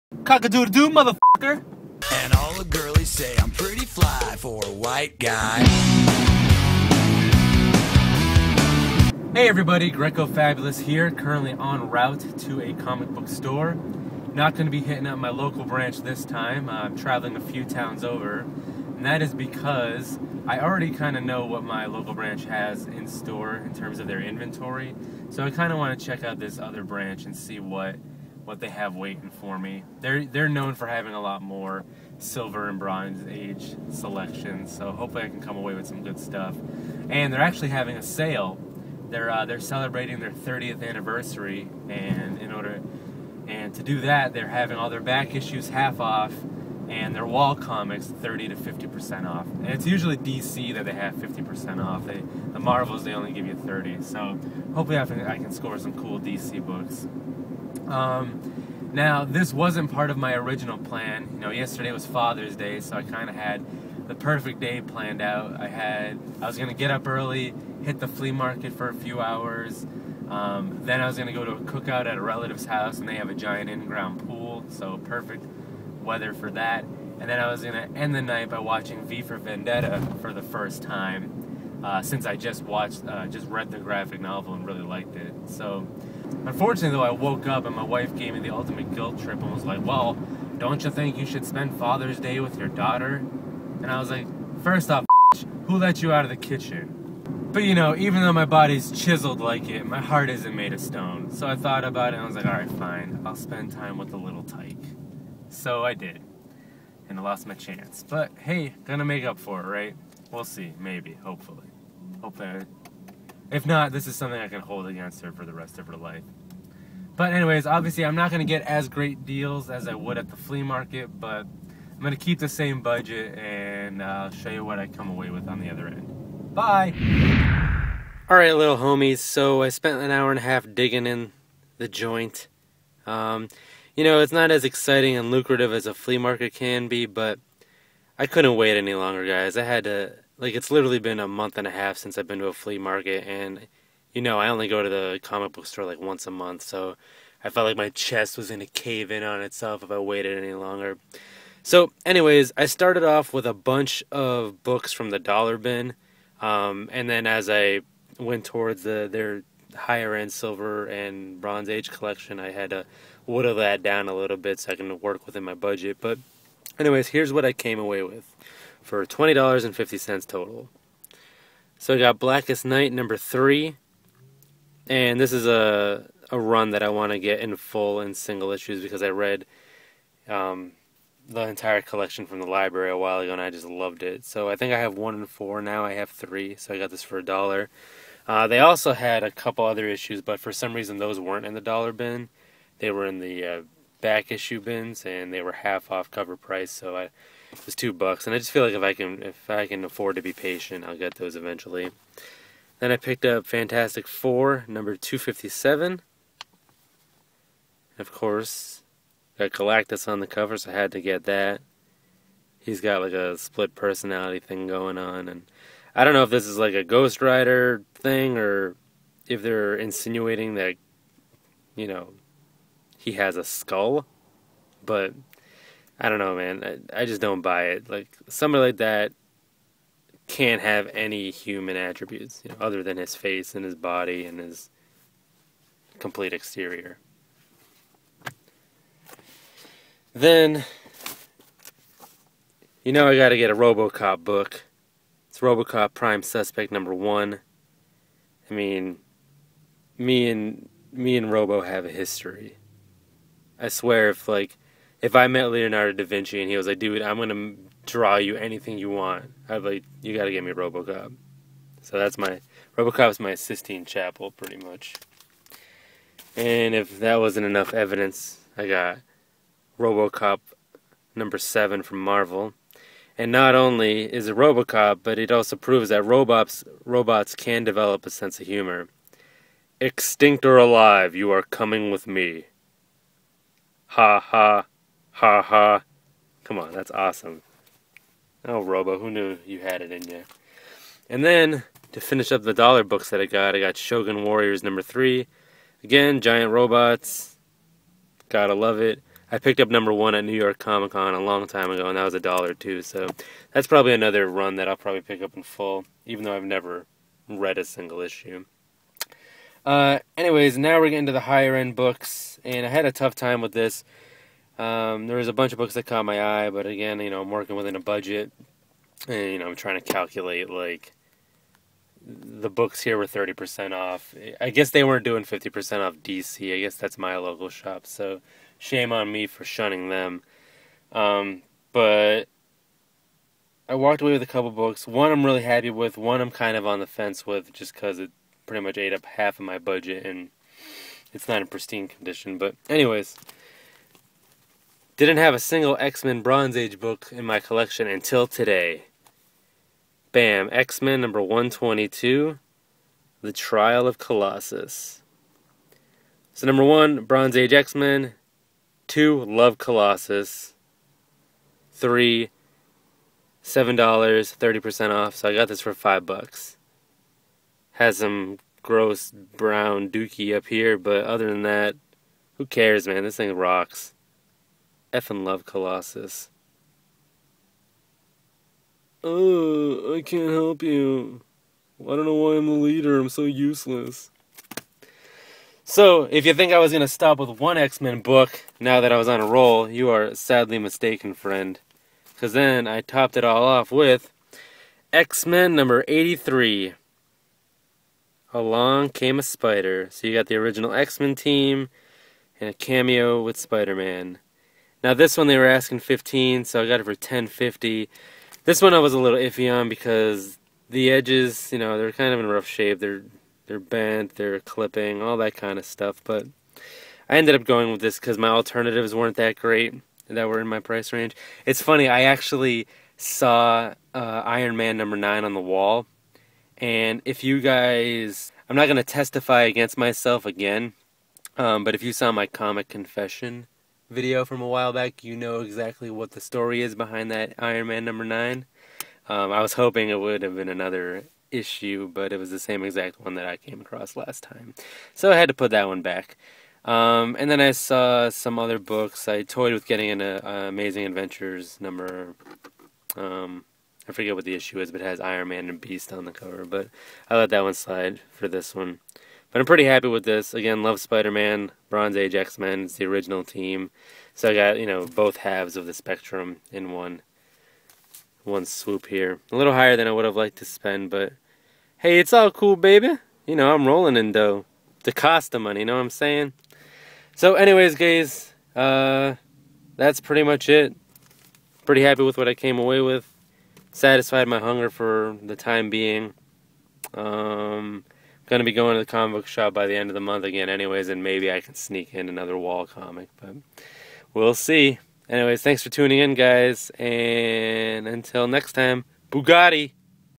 White People Be Shoppin' #4 - Comic Book Haul 6/17/13
Some books I picked up from my LCS.